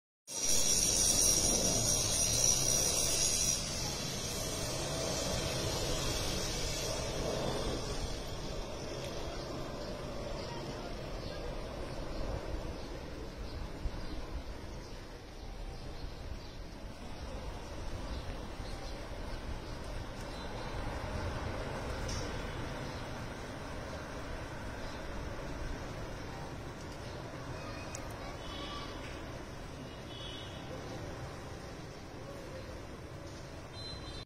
Chinese President Xi Jinping landed in Kathmandu, Nepal on a two-day state visit.
Chinese President Xi Jinping landed in Kathmandu on a two-day state to Nepal on Saturday afternoon.